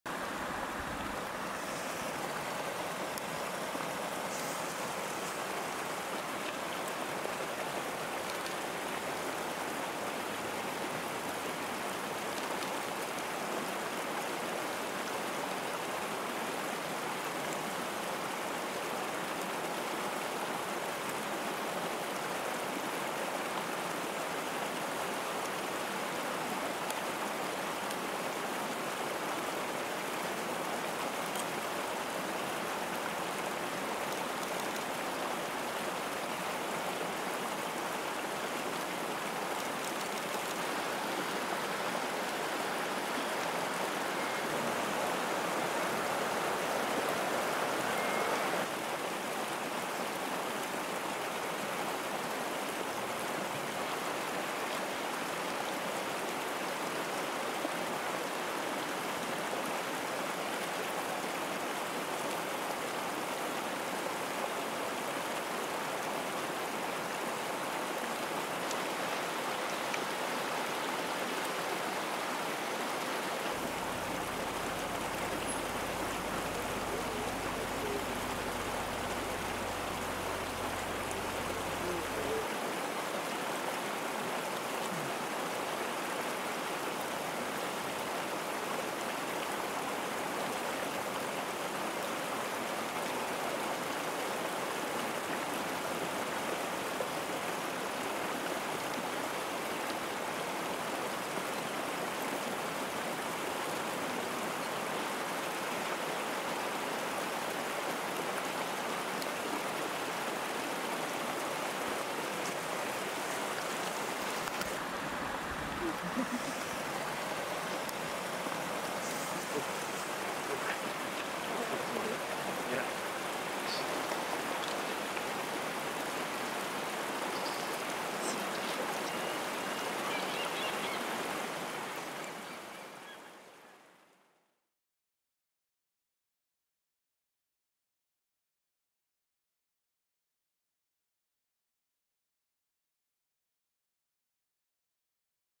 Wild Grizzly Watching Destination Bute Inlet Orford Bay
A grizzly bear in the distance on the river bank at the Bute Inlet, Orford bay Algard Creek.
The next one sitting in the water, holding the salmon with his paws and nibbling on it. It's still alive as the salmon tries to wiggle itself out of his paw. Two bald eagles fly past. Beautiful scenery all around.